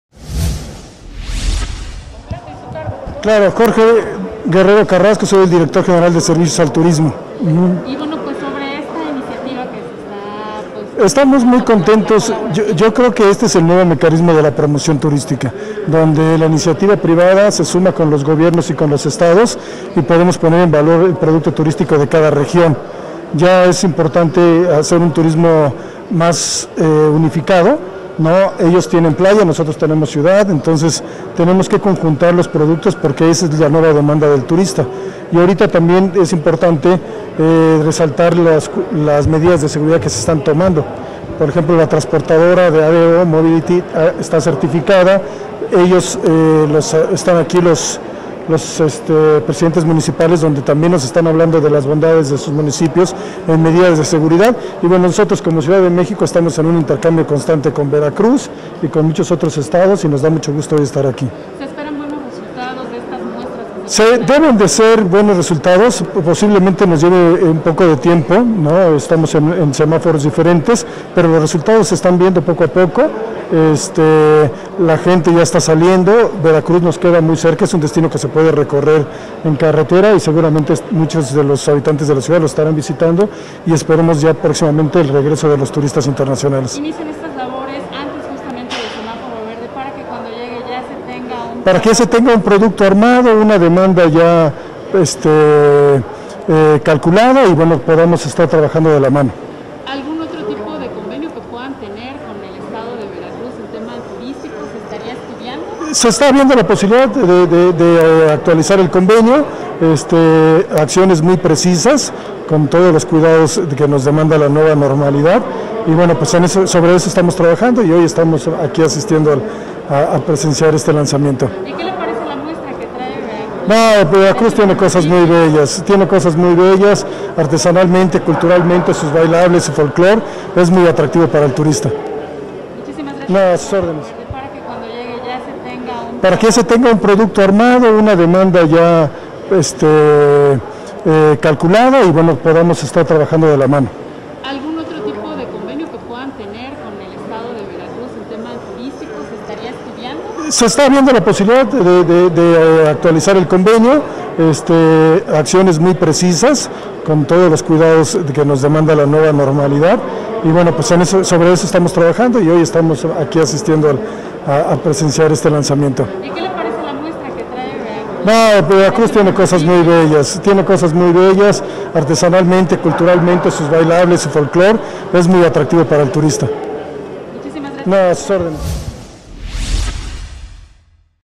Ven en Veracruz ejemplo de promoción “unificada y de intercambio”, en pandemia
El director general de Servicios al Turismo de la Secretaría de Turismo de la Ciudad de México, Jorge Guerrero Carrasco, afirmó que el nuevo mecanismo de la promoción turística es el que inicia el estado de Veracruz, haciendo sinergia con la iniciativa privada, los gobiernos y los Estados.